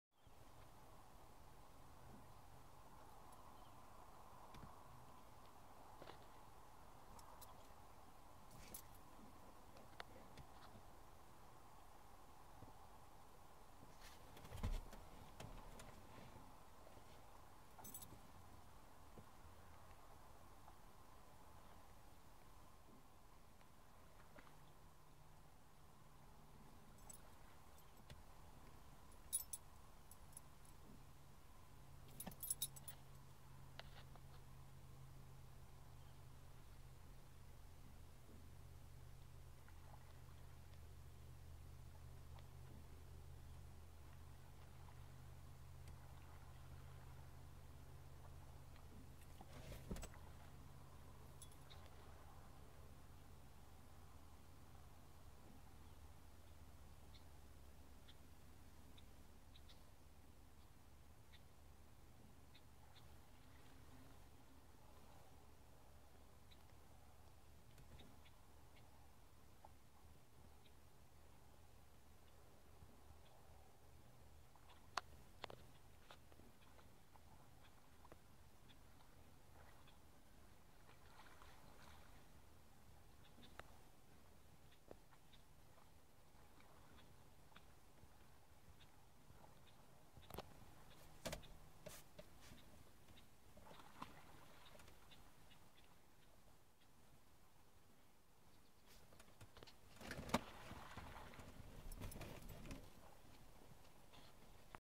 Dale saw this moose in Badger Basin, South Park, Park County, Colorado.
At first, Dale saw only the head and antlers sticking out of the water. He thought an elk was stuck in the pond. Then, the young bull moose emerged and allowed Dale to observe as it swam around and munched. We have seen baby moose with the parents on the Tarryall River and this is likely one of the local offspring - a young bull exploring his own territory.